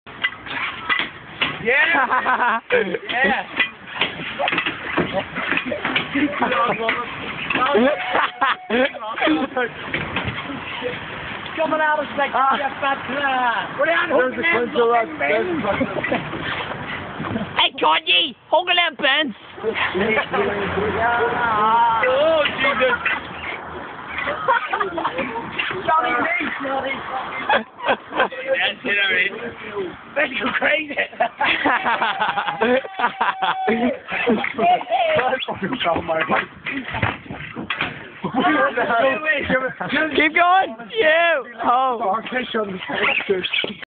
Advertisement Rodeo
A long long time ago (a month) in a galaxy far far away (round the corner of the union) two brave young warriors (two dickheads that got steamin off their tits) took on the almighty rodeo advertisement and survived its dastardly vibrations and movements to emerge victorious. this is their story